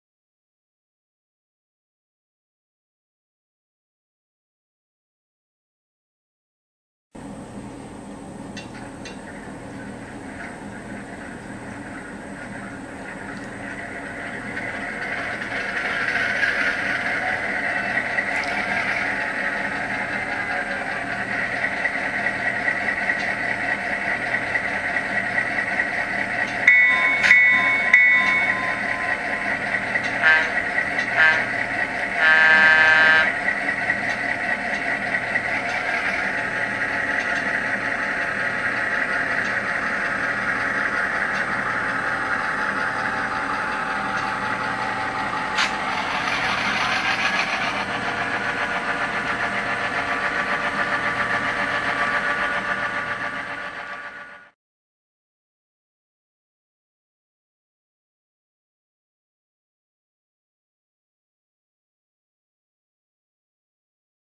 Kato NW2 with Tsunami sound decoder. First startup August 6, 2013
This is the startup of a Kato NW2 after the installation of a Tsunami decoder.  The frame had to be milled to allow for the decoder board and to allow for the passage of wires along the sides.  This was a very difficult installation, however, the results were well worth it.